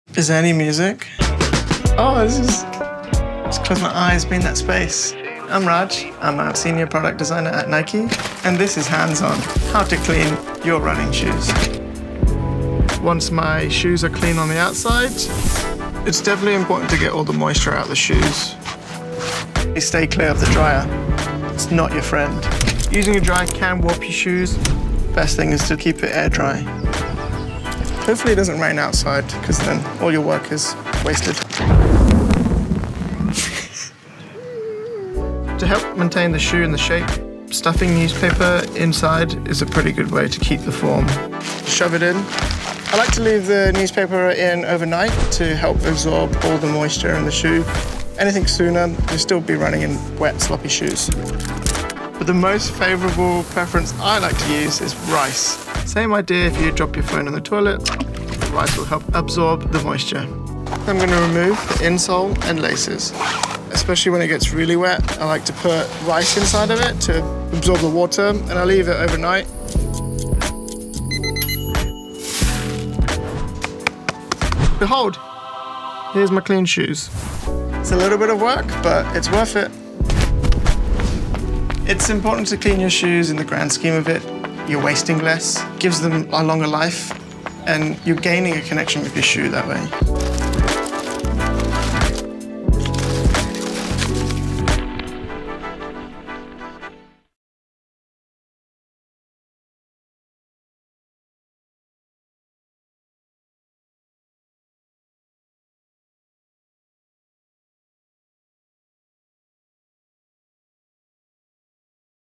How to Dry Your Running Shoes | Hands On (S2 E3) | Nike
Nike designer Raj knows that drying is a critical part of proper running shoe care. In this tutorial he goes deep on this shoe cleaning step, answering questions like, “can running shoes go in the dryer?” Why take the time to properly dry? It will help your running shoes last longer and keep them smelling fresh. Have your own tips to increase running shoe mileage? We’d love to hear, so leave them in the comments!

00:04 Meet Raj!
00:19 Can Shoes Go in the Dryer?
00:35 Basic Drying Tip
00:53 Raj’s Pro Drying Tip
01:17 Why Take the Time?

Want more sustainably minded running shoe care? Check out 12 tips from Nike designers and elite runners